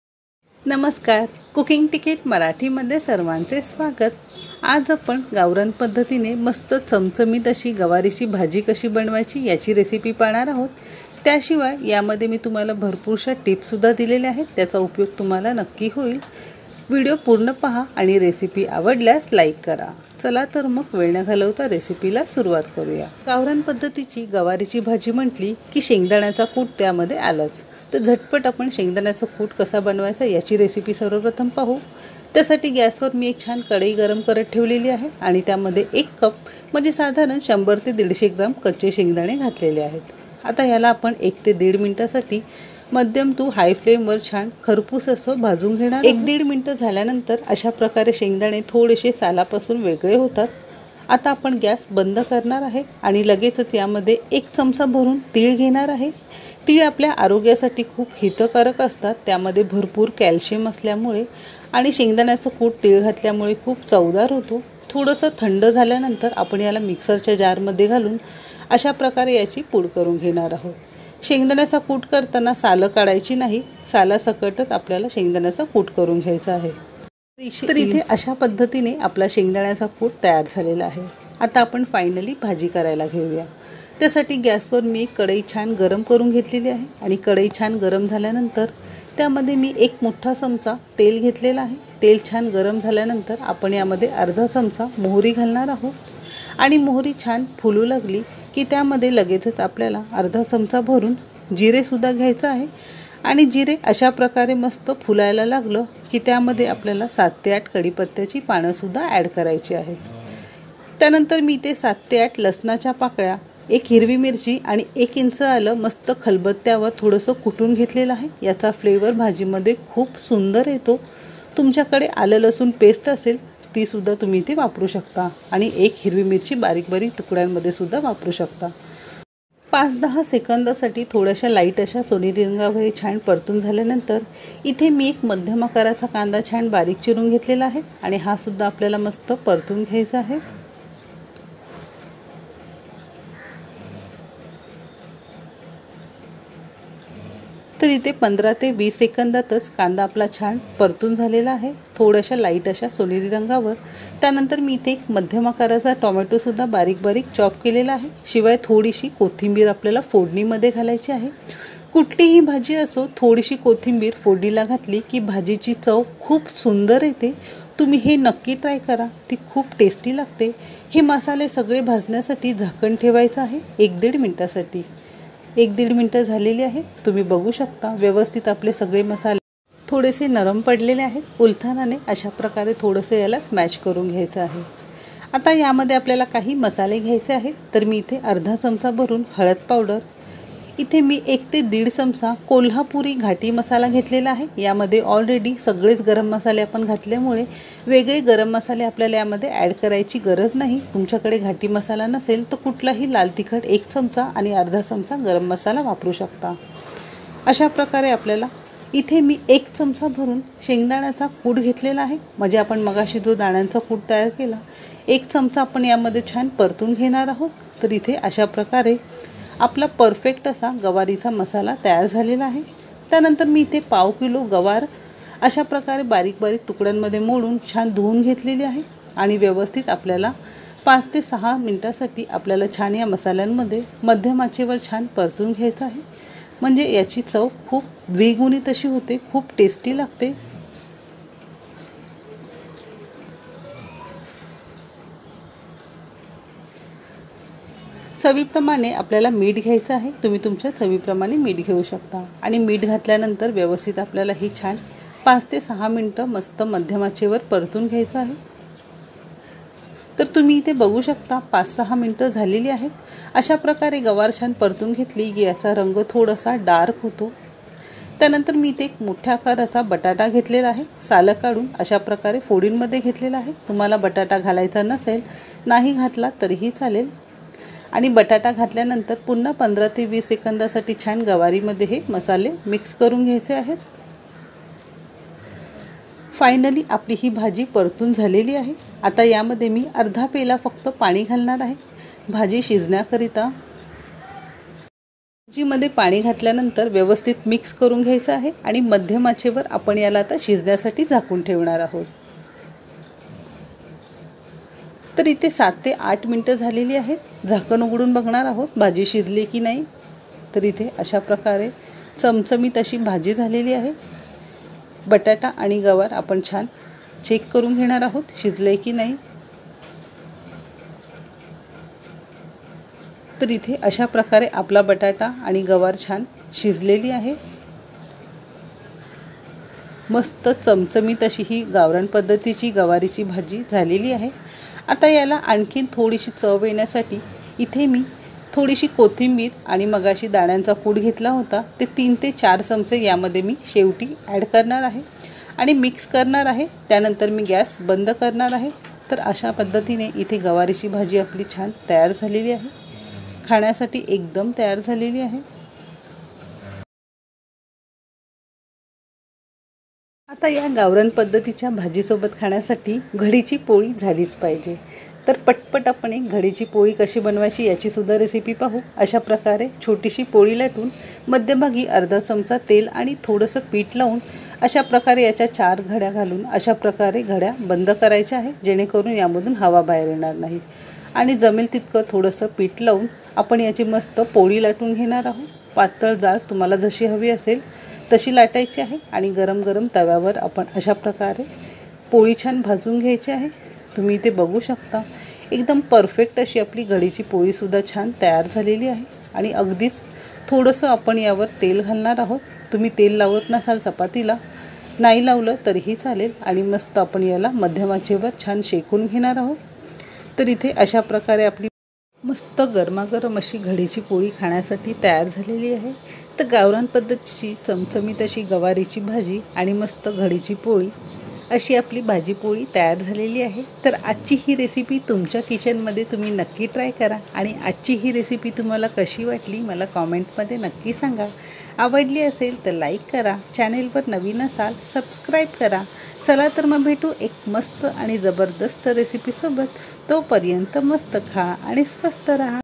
गावरान पध्दतीने चमचमीत व चवदार गवारीची भाजी , एकदा नक्की बनवा सारखे बनवाल 😋👌
साहित्य / ingrediants
शेंगदाण्याचा कूट बनवण्यासाठी👇
* शेंगदाणे  - 1कप ( 150 ग्राम )
* तीळ -1 चमचा ( 20 ग्राम )
भाजुन जाडसर कुट करुन घेणे.

गावरान गवरीची भाजी बनवण्यासाठी साहित्य.👇
* गवार - पाव किलो ( 250 ग्राम ) मोडूनघेणे .
* बटाटा - 1 मोठा ( फोडी करुन )
* शेंगदाणे कुट - 5 चमचे
* आलं लसूण हिरवी मिरची पेस्ट  - 2 चमचे
* तेल - 1 मोठा चमचा
* मोहरी - ½चमचा
* जिरे- ½ चमचा
* कडिपत्ता - 7/8 पाने
* कांदा - 1 मध्यम आकाराचा ( बारिक चिरलेला )
* टोमॅटो - 1 मध्यम आकाराचा ( बारिक चिरलेला )
* कोथिंबीर - मुठभर  ( बारिक चिरलेली)
* हळद पावडर - ½ चमचा
* कोल्हापुरी घाटी मसाला- ( कुठलही लाल तिखट)          1 ½ चमचा
* मीठ - चवीनुसार
* पाणी - ½ पेला  ( गरजेनुसार )